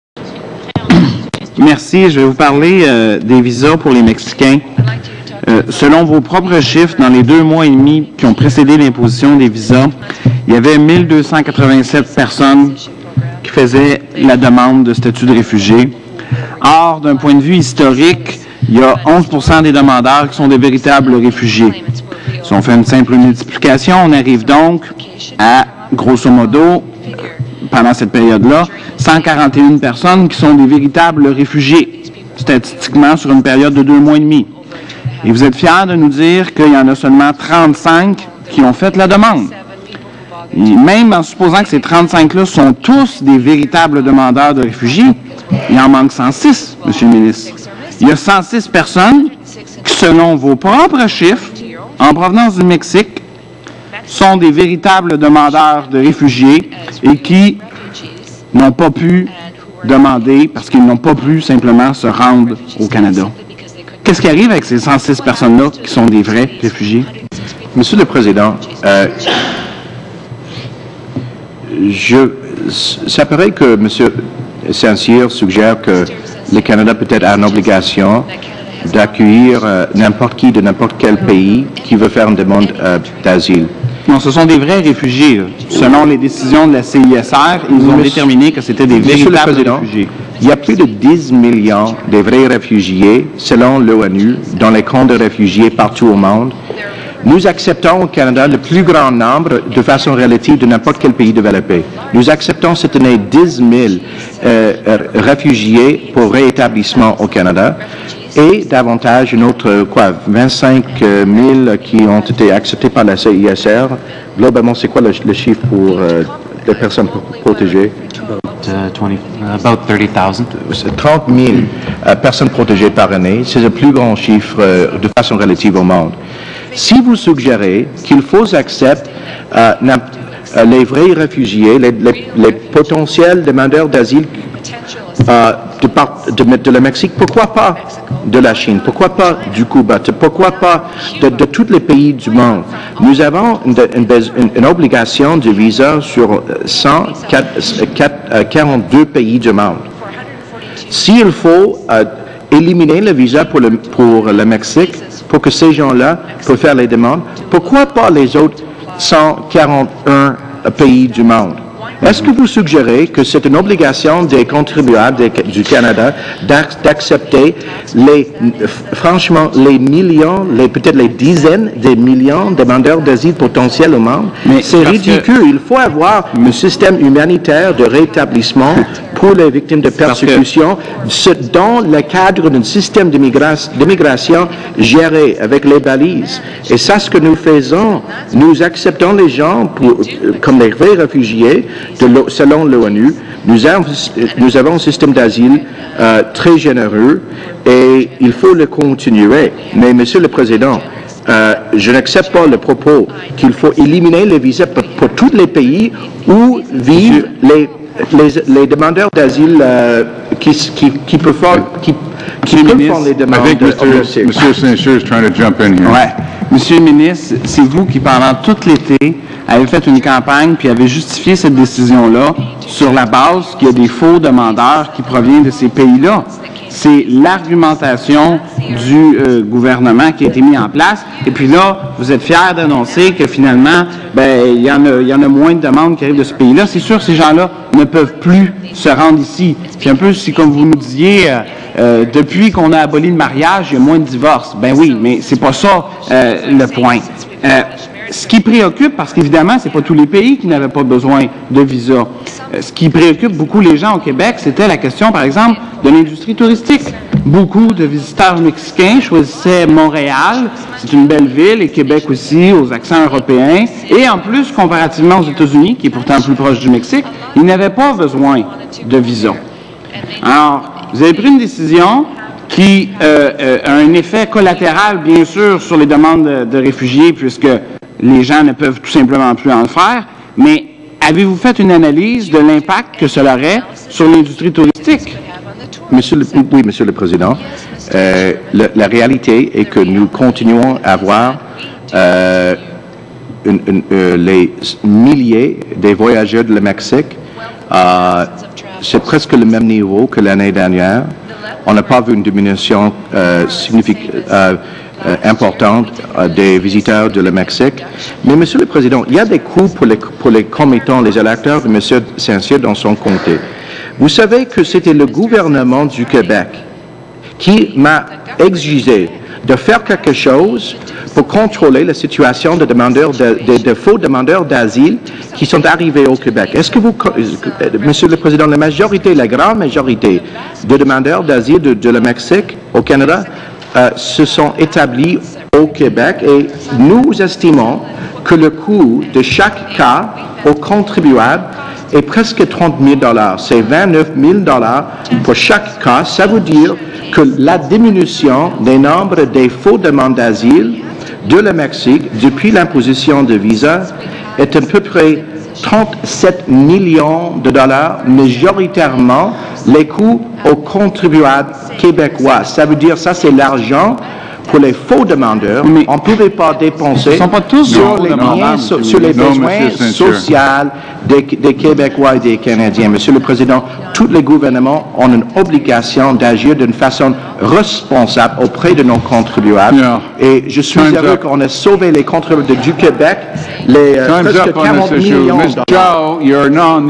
Comité - Échange avec le ministre concernant l'imposition de visas aux Mexicains
Thierry St-Cyr, député de Jeanne-Le Ber et porte-parole du Bloc Québécois en matière de citoyenneté et immigration échange avec le ministre concernant sa décision d'imposer des visas aux touristes mexicains